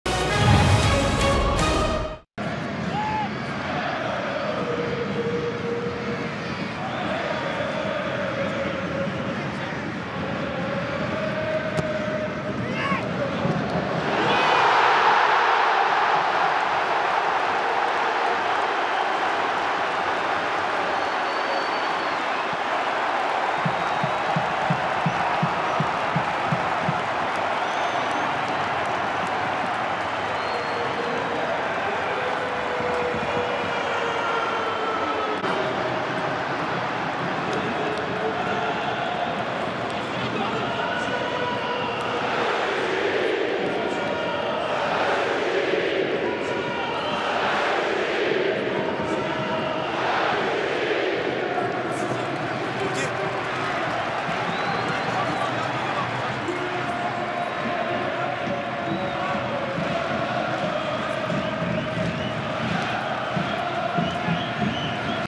But Mathieu DEBUCHY (74') / AS Saint-Etienne - Girondins de Bordeaux (3-0) (ASSE-GdB)/ 2018-19
AS Saint-Etienne - Girondins de Bordeaux ( 3-0) But de Mathieu DEBUCHY (74'). Tous les buts de AS Saint-Etienne - Girondins de Bordeaux en vidéo.

Ligue 1 Conforama - Saison 2018/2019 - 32ème journée Stade Geoffroy-Guichard - dimanche 14 avril 2019

Buteurs : Wahbi KHAZRI (56' pen - AS Saint-Etienne) / Mathieu DEBUCHY (74' - AS Saint-Etienne) / Mathieu DEBUCHY (90' - AS Saint-Etienne)